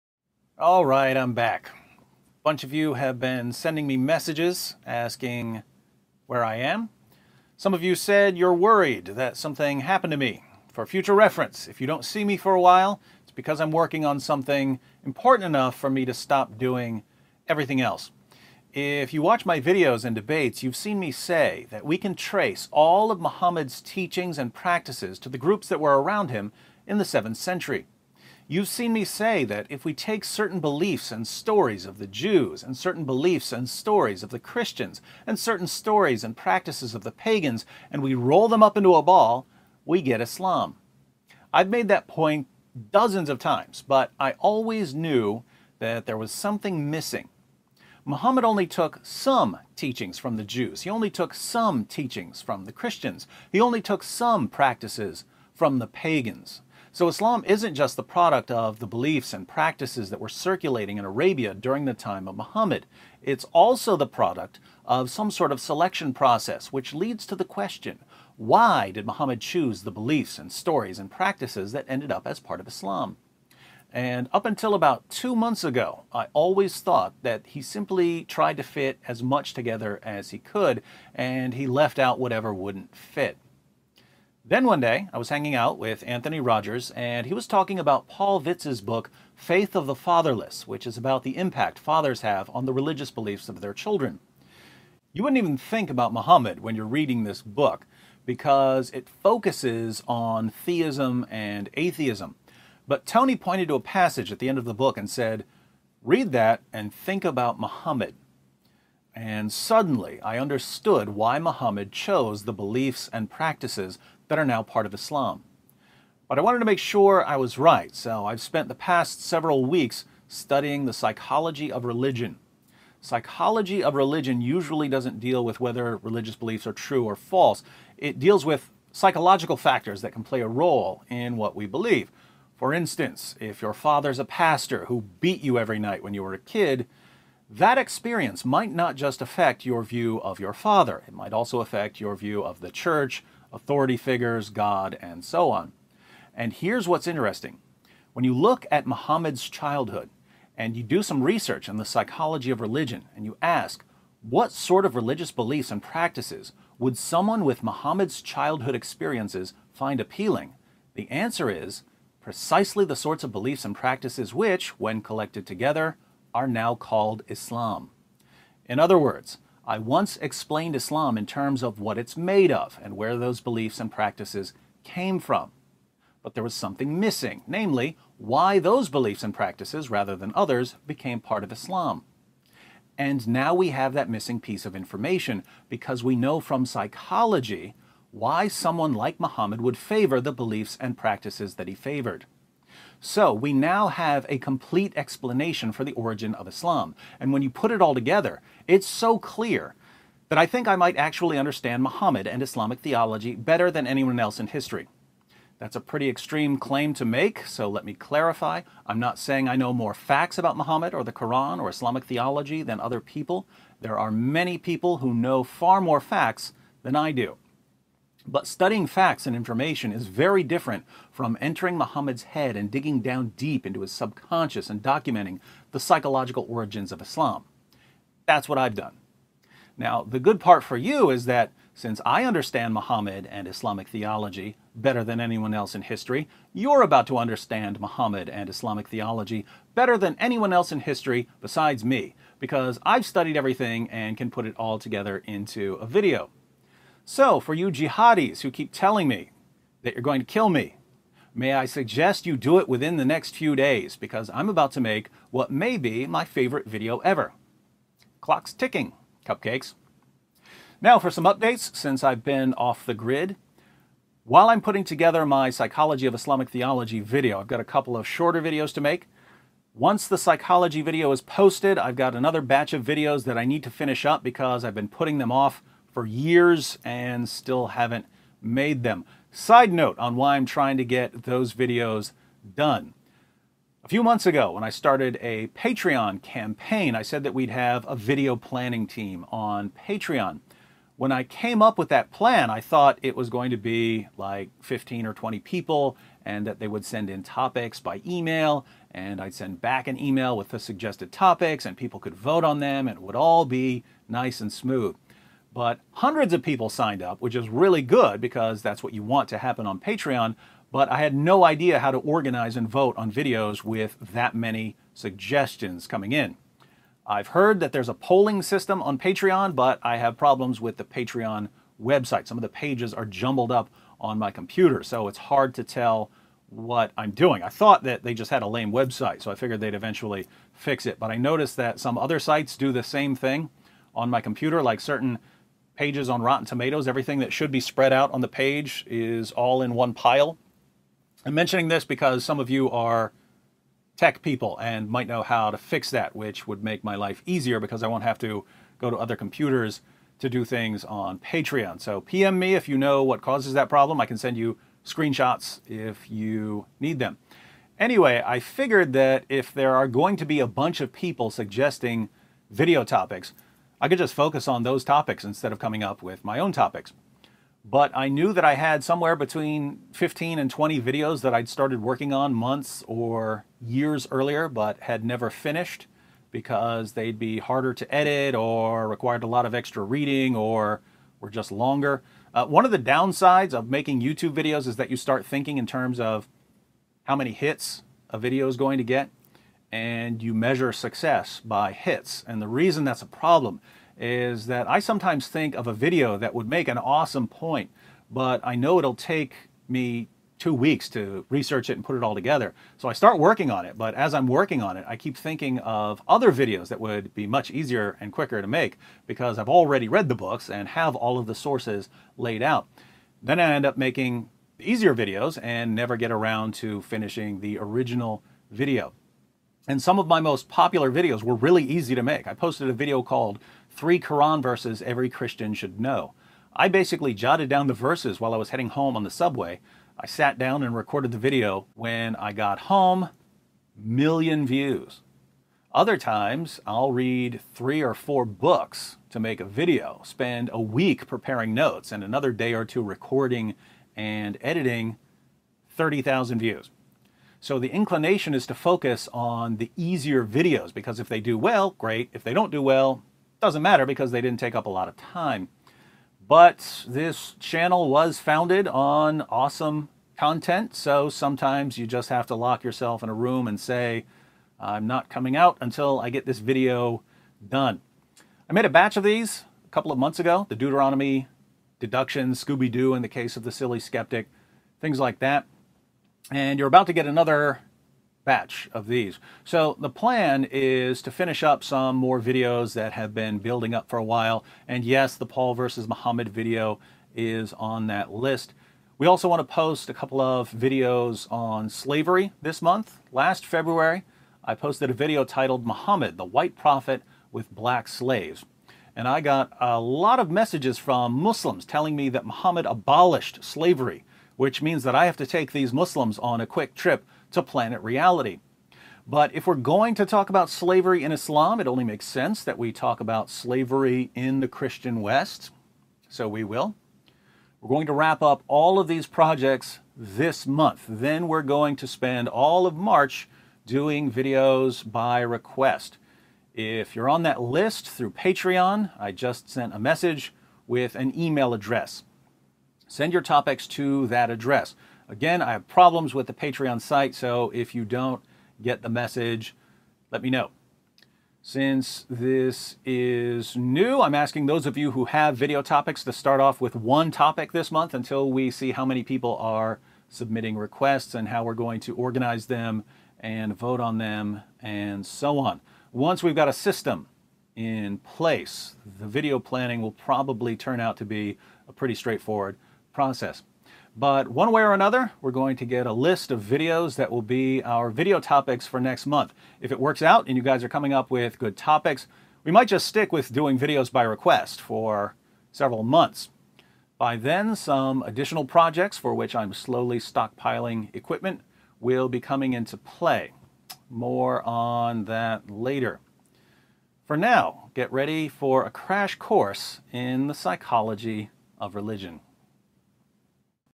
I'M BACK
=== Original Description ===

I've been off the grid for a while, studying up for what may be my favorite video ever. I've got all of the studying done, so I'll have the video put together in a few days. Here's a quick update on everything for those of you who've been asking where I am.

Statistics of original video: 📅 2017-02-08  ⏱ 9:54  ▶ 130803  👍 7618  💬 1265  🆔 RVN3R_kl73k